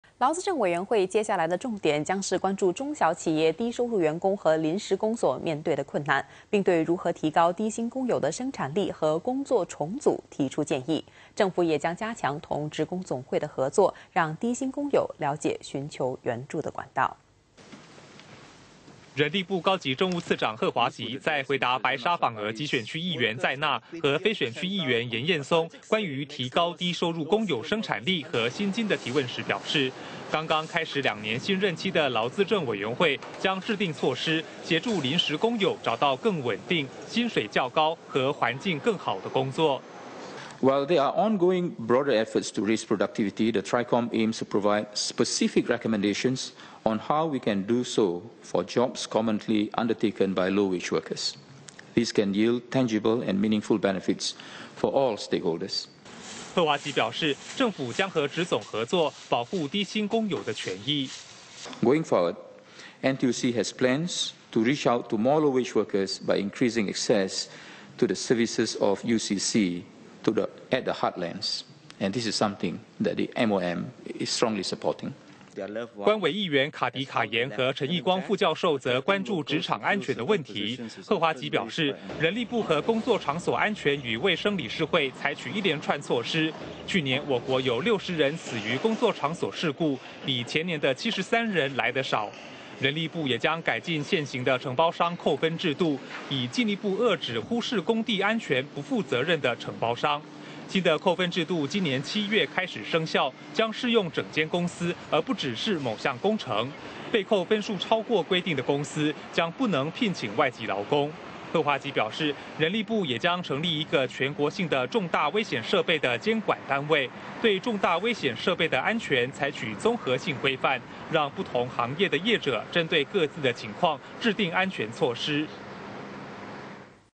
劳资政委员未来将关注中小企业低薪工友所面
劳资政委员会接下来的重点将是关注中小企业低收入员工和临时工所面对的困难，并对如何提高低薪工友的生产力和工作重组提出建议。政府也将加强同职工总会合作，让低薪工友了解寻求援助的管道。